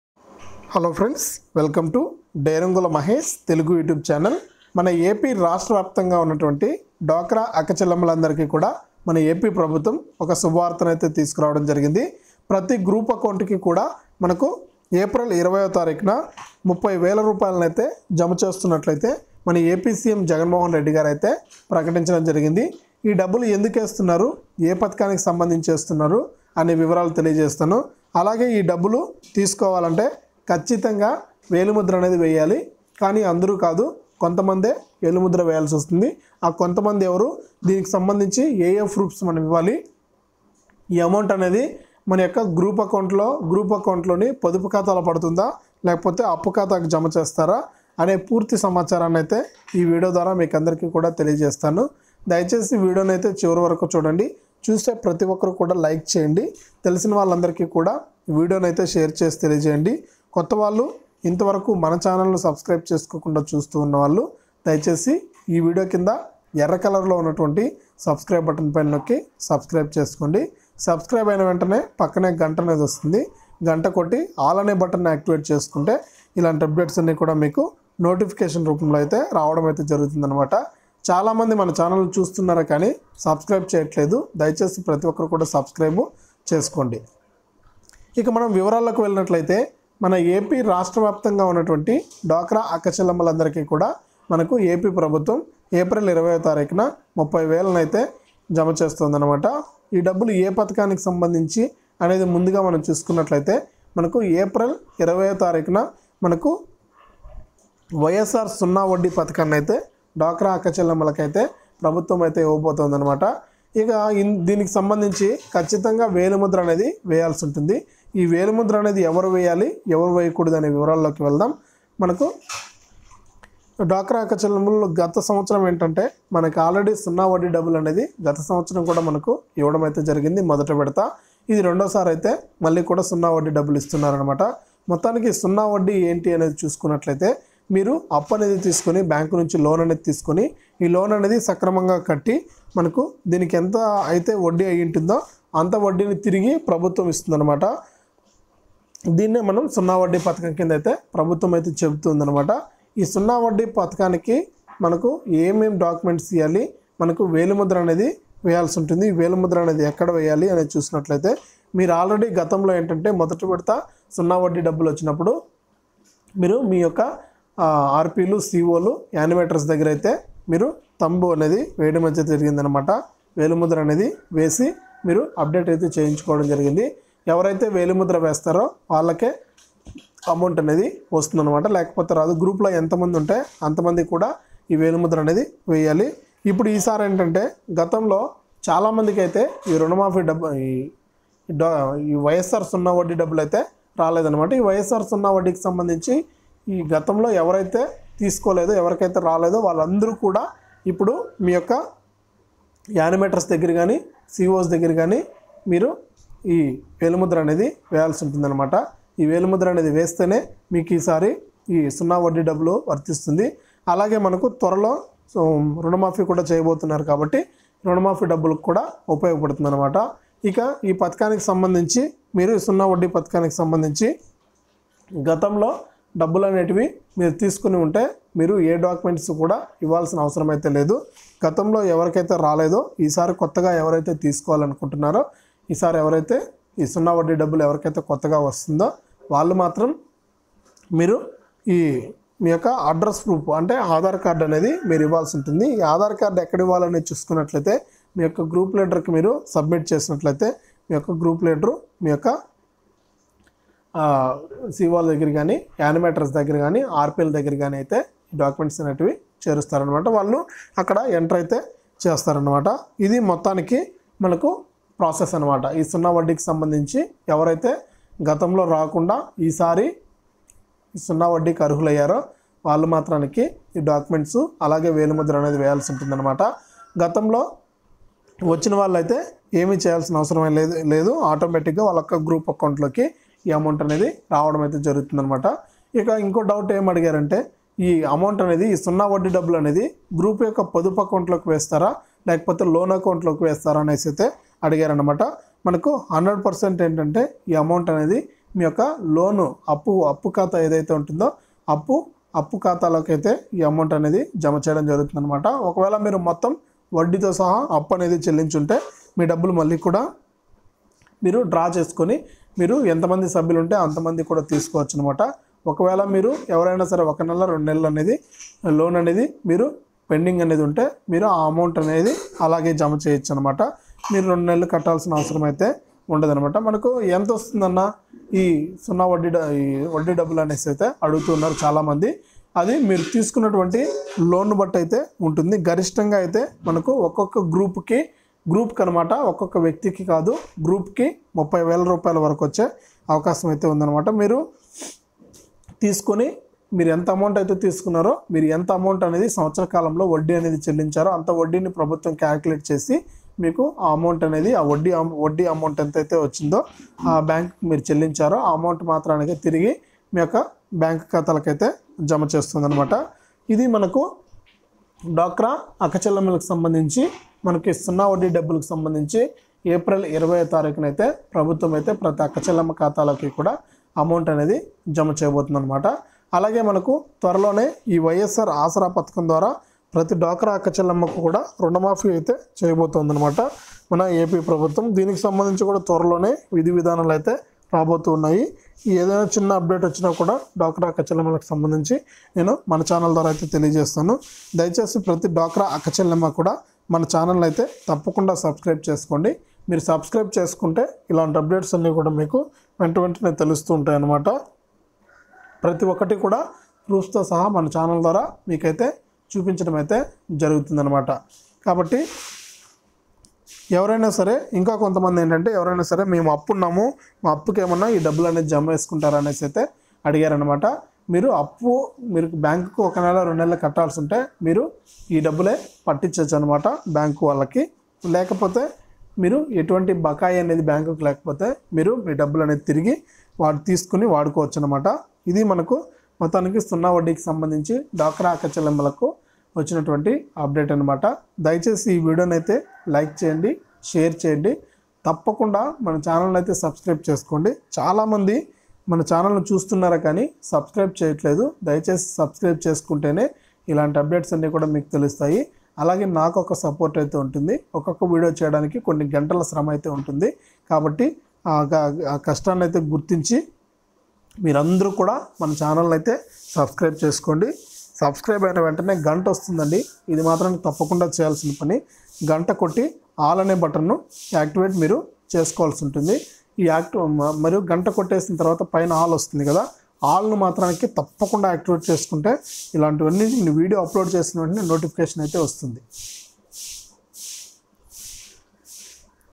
ఏపీ డ్వాక్రా మహిళలకు ఏప్రిల్ 20న 30000|రేపటినుండి వేలిముద్రలు వేయాలి|DWACRA YSR SUNNA VADDI SCHEME
ఏపీ డ్వాక్రా మహిళలకు ఏప్రిల్ 20న 30000|రేపటినుండి వేలిముద్రలు వేయాలి|DWACRA YSR SUNNA VADDI SCHEME@DERANGULAMAHESH